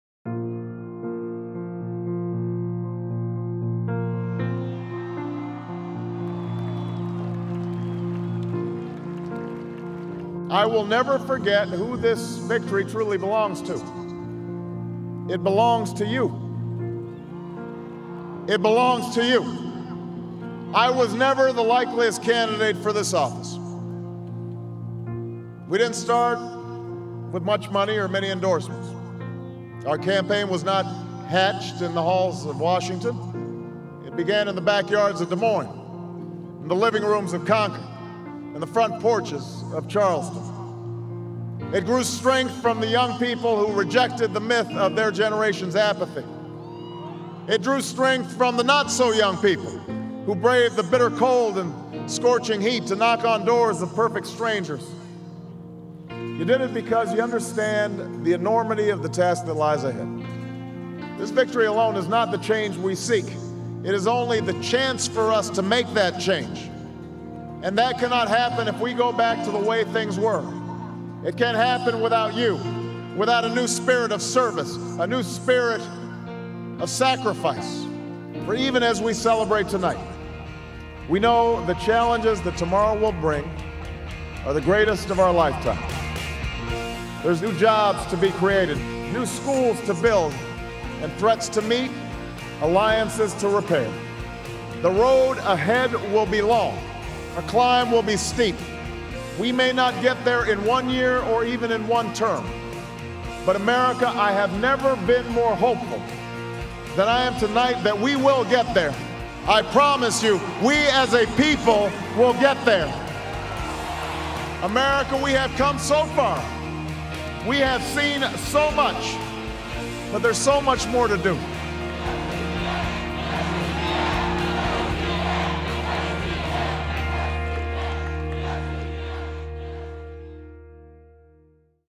2012 Presidential Election: Volunteer for Barack Obama's Campaign
We've accomplished so much since Election Day 2008, but let's make sure we finish the job together in 2012. Election Day 2012 is exactly one year from this week. That's why we're not waiting to build this campaign. Watch the video, then commit to volunteer and say you'll be with us in 2012.

Since Election Day 2008, we've:

• Saved over one million jobs in the U.S. auto industry

• 20 consecutive months of job creation

• Took the fight to Al-Qaeda

• Reduced and secured nuclear weapons

• Passed health reform, lowering costs and insuring 32 million

• Ended "Don't Ask, Don't Tell":

• Nominated first Latina to Supreme Court

• Doubled college grants, now for 9 million students

• New Protections against credit card companies

• New 54.5 MPG fuel standards, saving $8,200 per family

• Reined in Wall Street --ended taxpayer bailouts

• Ending the war in Iraq

Excerpts from Barack Obama's 2008 Presidential Election Night acceptance speech:

"I will never forget who this victory truly belongs to -- it belongs to you. It belongs to you.

I was never the likeliest candidate for this office. We didn't start with much money or many endorsements. Our campaign was not hatched in the halls of Washington -- it began in the backyards of Des Moines and the living rooms of Concord and the front porches of Charleston.

It grew strength from the young people who rejected the myth of their generation's apathy. It grew strength from the not-so-young people who braved the bitter cold and scorching heat to knock on the doors of perfect strangers.

You did it because you understand the enormity of the task that lies ahead.
This victory alone is not the change we seek -- it is only the chance for us to make that change. And that cannot happen if we go back to the way things were. It cannot happen without you --without a new spirit of service, without a new spirit of sacrifice. For even as we celebrate tonight, we know the challenges tomorrow will bring are the greatest of our lifetime.

There jobs to be created, new schools to build, and threats to meet --alliances to repair. The road ahead will be long, the climb will be steep. We may not get there in one year or even in one term, but America I have never been more hopeful than I am tonight that we will get there. I promise you we as a people we'll get there. America we have come so far we have seen so much but there's so much more to do."